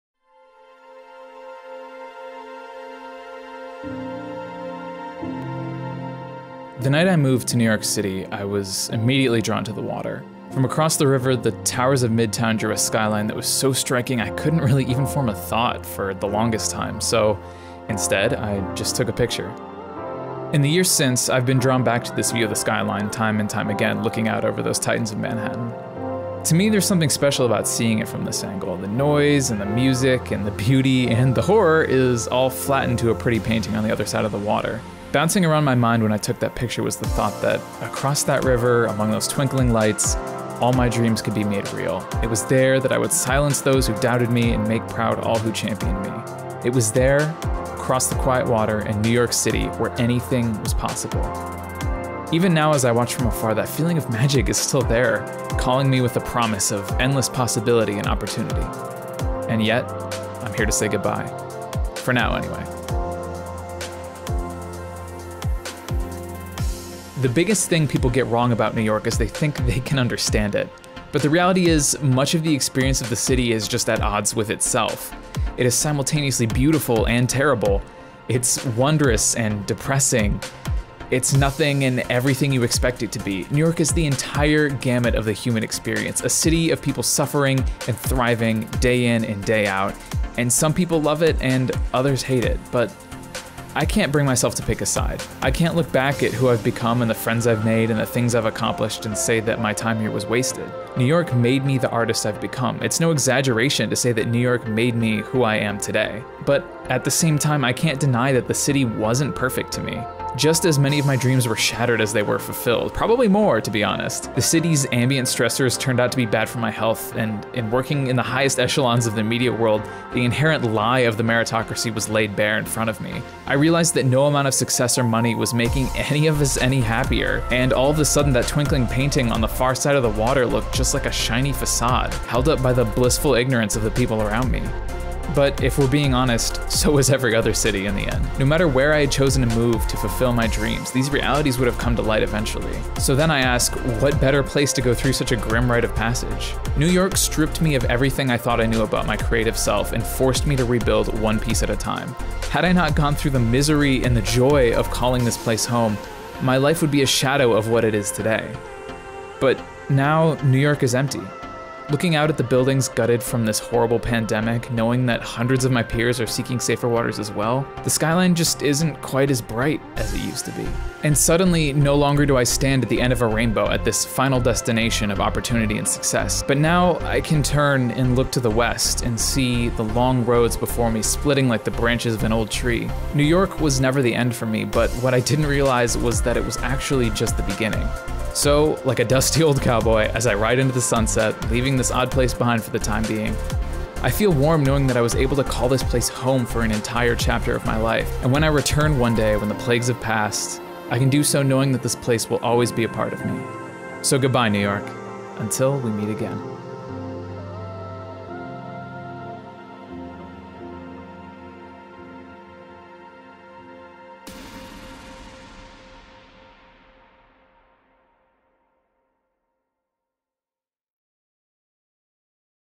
ARTIST LOG 002: Leaving New York City
The night I moved to New York City, I was immediately drawn to the water. From across the river, the towers of midtown drew a skyline that was so striking, I couldn’t really even form a thought for the longest time. So, instead, I just took a picture.

In the years since, I’ve been drawn back to this view of the skyline time and time again, looking out over the titans of Manhattan. To me, there’s something special about seeing it from this angle. The noise and music and beauty and horror of it all is flattened to a pretty painting on the other side of the water.

Bouncing around my mind when I took that picture was the thought that, across that river, among the twinkling lights, all my dreams could be made real. It was there where I would silence those who doubted me and make proud all who’d championed me. It was there, across the quiet water, where anything was possible. Even now as I watch from afar, that feeling of magic is still there, calling me with the promise of endless possibility and opportunity.

And yet, I’m here to say goodbye. For now, anyway.

The biggest thing people get wrong about New York is that they think they can understand it. But the reality is, much of the experience of the city is at odds with itself.

It is simultaneously beautiful and terrible. It’s wondrous and depressing. It is nothing and everything you expect it to be. New York is the entire gamut of the human experience. A city of people suffering and thriving day in and day out. Some love it, others hate it.

But I can’t bring myself to pick a side. New York was never forever for me and I knew, sooner or later, I’d be on my next adventure.

I can’t look back at who I’ve become and the friends I’ve made and the things I’ve accomplished and say that my time here was wasted. New York made the artist I’ve become. It’s no exaggeration to say New York made me who I am today.

But, at the same time, I can’t deny that the city wasn’t perfect to me. Just as many of my dreams were shattered as they were fulfilled. The city’s ambient stressors turned out to be bad for my health. In working in the highest echelons of the media world, the inherent lie of the meritocracy was laid bare. I realized that no amount of success or money was making any of us any happier and all of the sudden that twinkling painting on the far side of the water looked like just a shiny facade, held up by the blissful ignorance of my peers. I quickly learned that New York City is a place where hopeful people are caught beneath the boots of the wealthy and connected.

But so is every other city, in the end. No matter where I had chosen to move, these realities would have come to light eventually. So then, what better place to go through such a grim rite of passage?

New York stripped me of everything I thought I knew about my creative self and forced me to rebuild one piece at a time. Had I not gone through the joy and misery of calling this place home, my life would be a shadow of what it is today.

But now New York is empty. Looking out at the buildings gutted from a horrible pandemic, knowing that hundreds of my peers are seeking safer waters as well, the skyline isn’t quite as bright in my mind’s eye.

No longer do I stand at the end of the rainbow, at the final destination of opportunity and success.

Now I can look to the west and see the long roads before me splitting like the branches of an old tree. New York was never the end. But what I didn’t realize was that it was actually just the beginning.

So as I ride into the sunset, leaving this odd place behind for the time being, I feel warm knowing I was able to call this place home. And when I return one day when the plagues have passed, for work and for friends, I can do so knowing that this place will always be a part of me.

So goodbye, New York. Until we meet again.